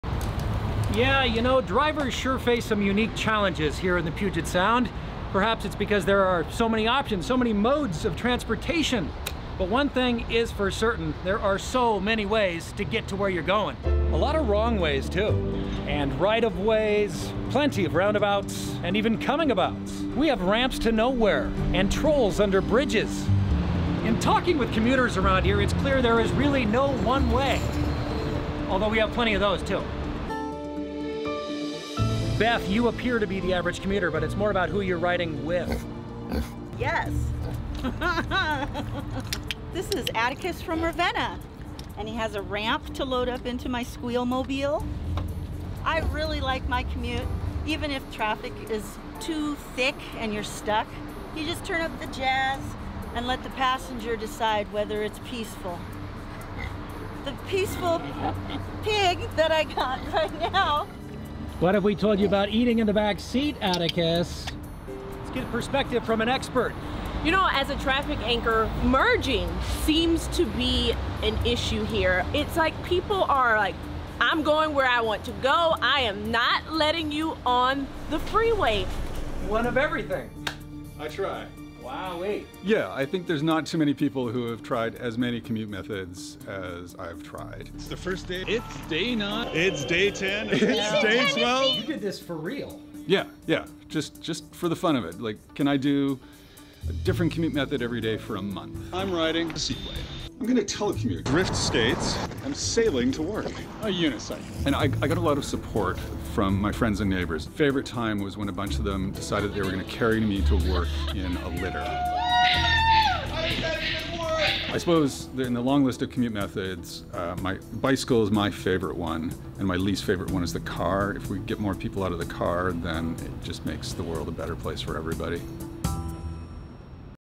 How do you commute? The creative ways Seattle workers are beating the backups
As more people are headed back to the office, some commuters will try anything to avoid getting stuck in traffic.